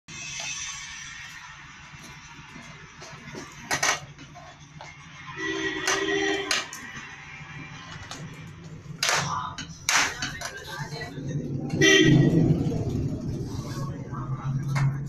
CUTE PUPPY PLAYING TIME # SHORT
BORDER COLLIE PUPPY ARE PLAYING WITH SLEEPER